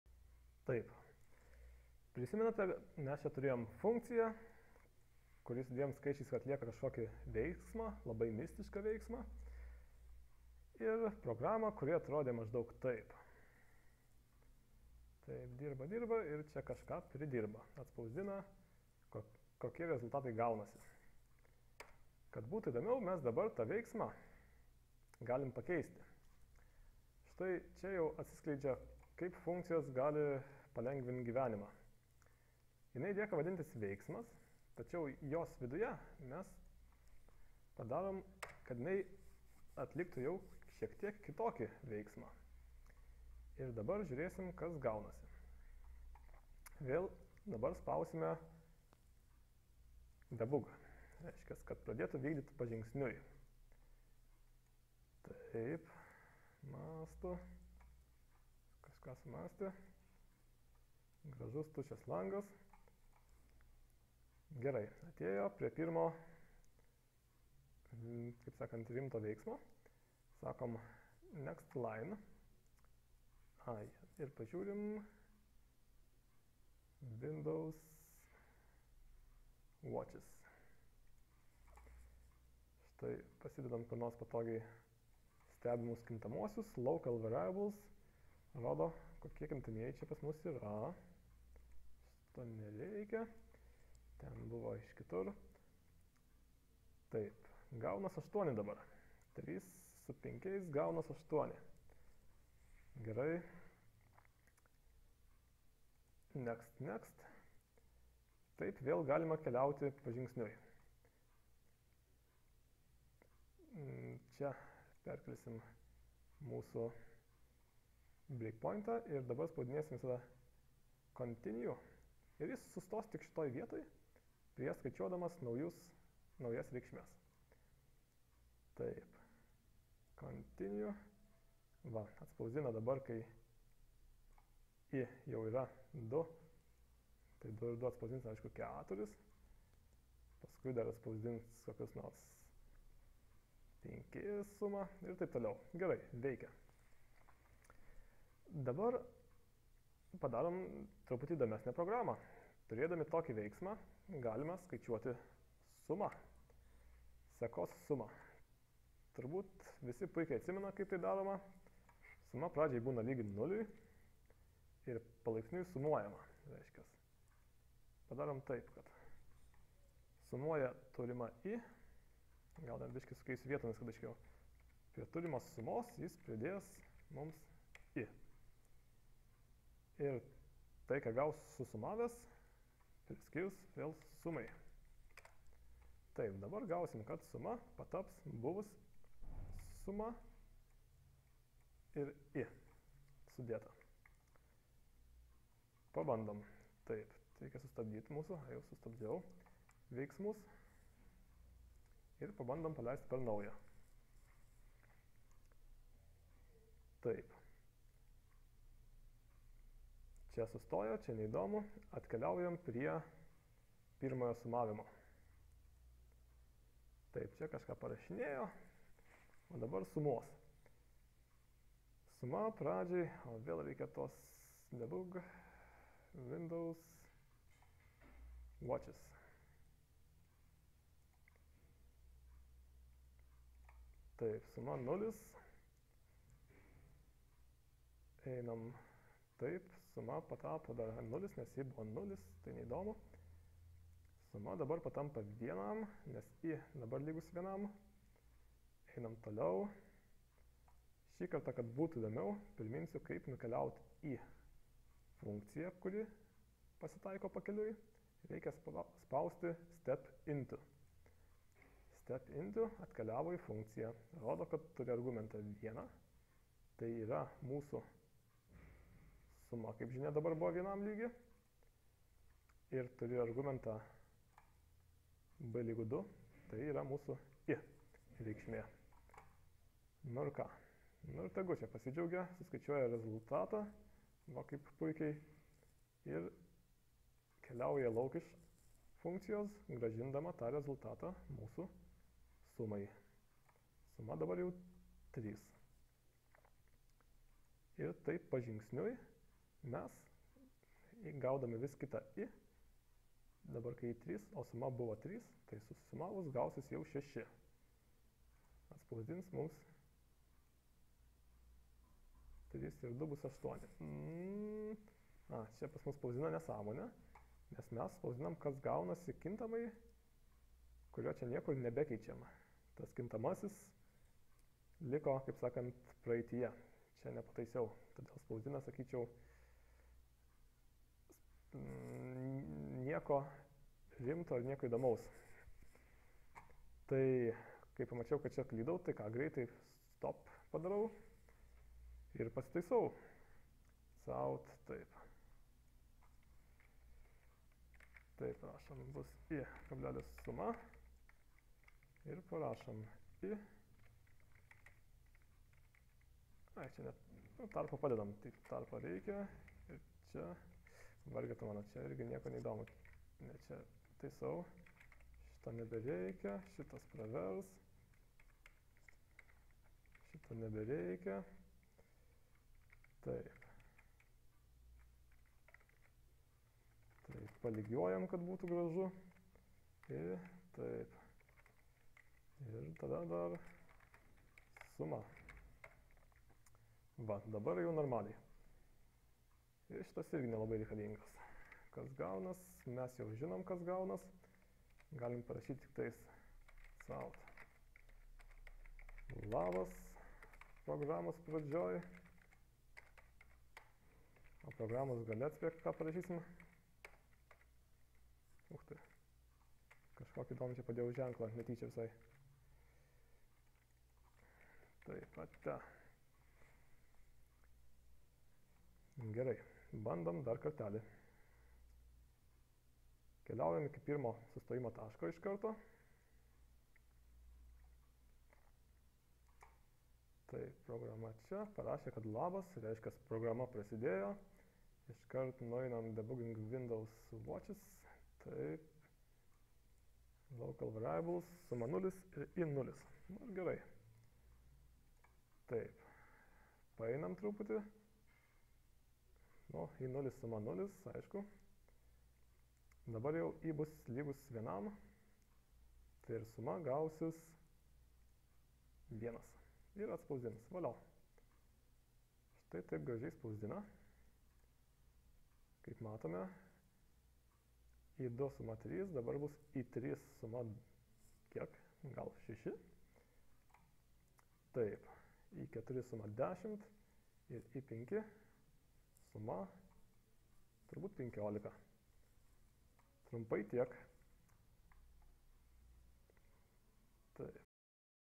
C++ programos veiksmų sekimas 2 - sumos skaiciavimas
C++ programos veiksmų sekimas (debug). Funkcijos iškvietimas ir pan. - sumos skaiciavimas